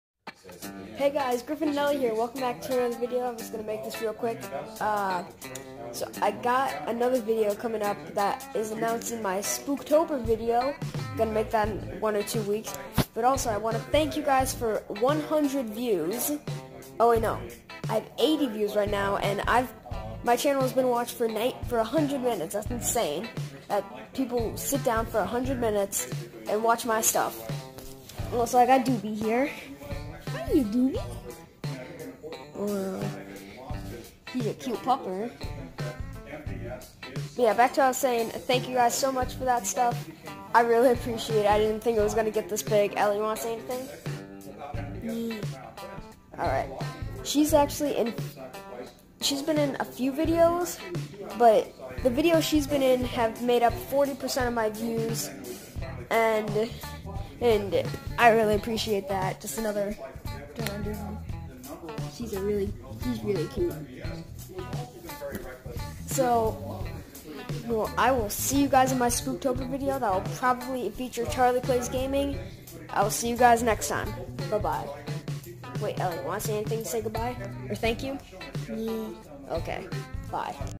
Thank you all! Feat. Elli and the surprise pupper
Thank you all so much for 100 viewing minutes! I'm so lucky to have such incredible people watching my videos!